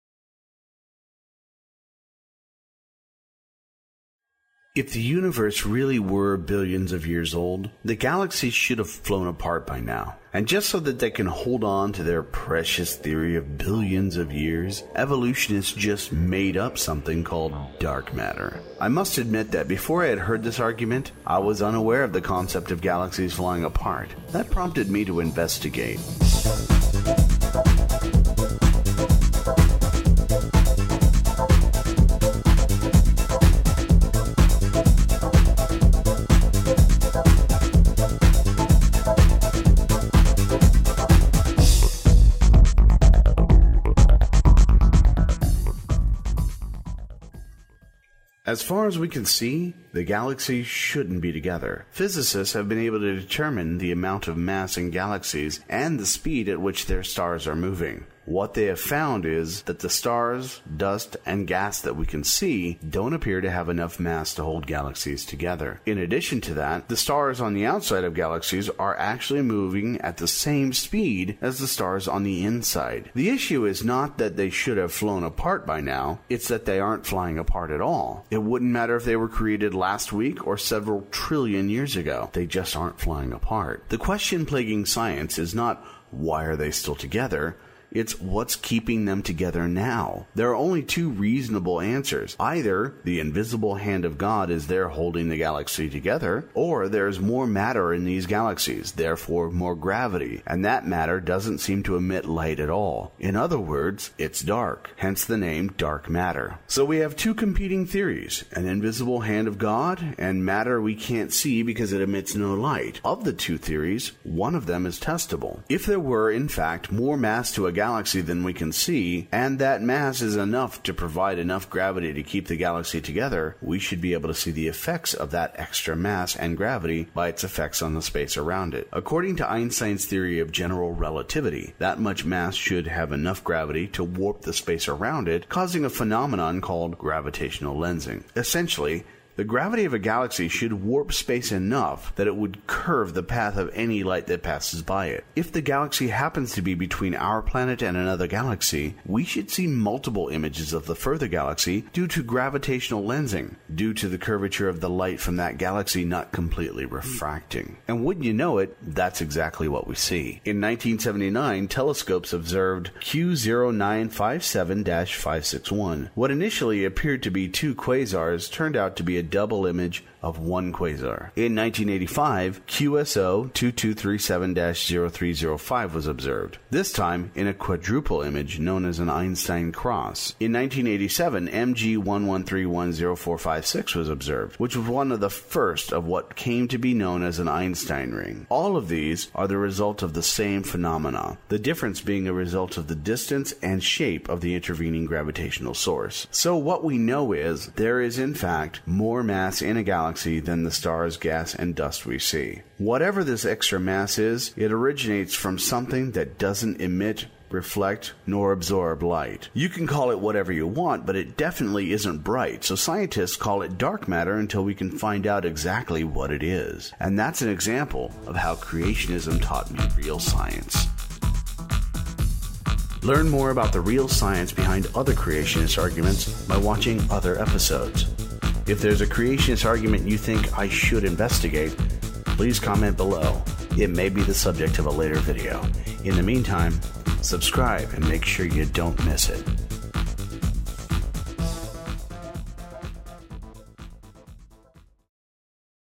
How Creationism Taught Me Real Science 02 Dark Mat
This chapter examines the science and motivations behind the evolutionist concept of dark matter.